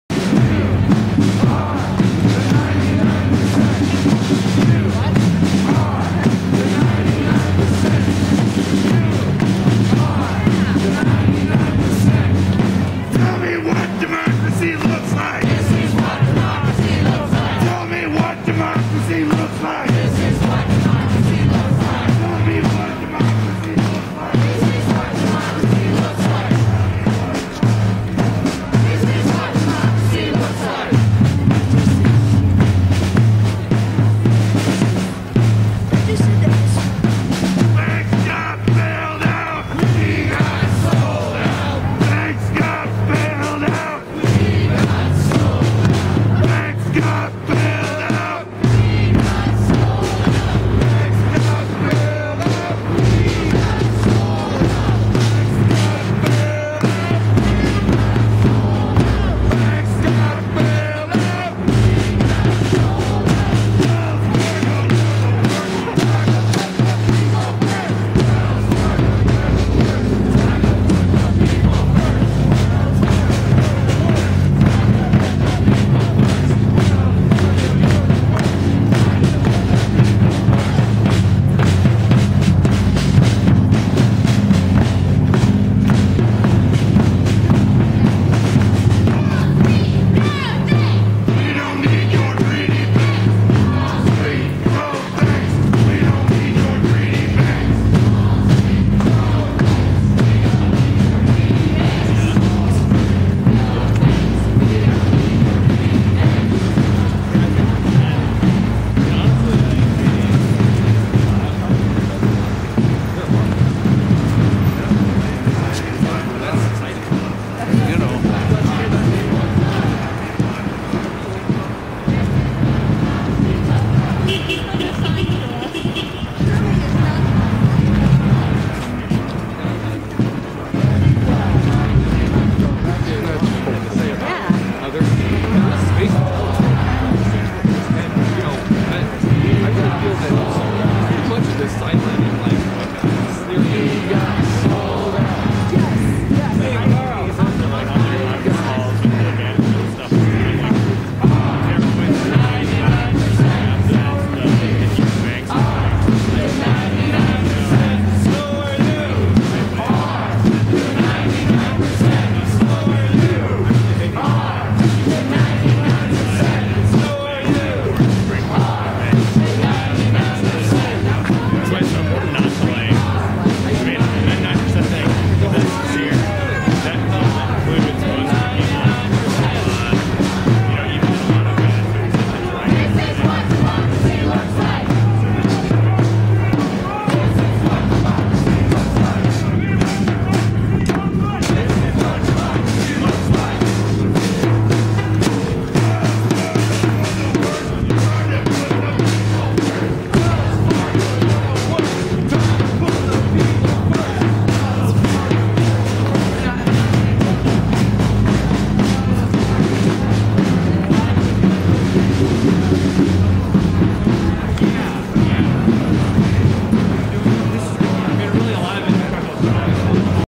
Occupy Iowa City Oct 29, 2011 Part 2 CGP to Ped Mall
March starts at College Green Park, heads to ped mall in downtown Iowa City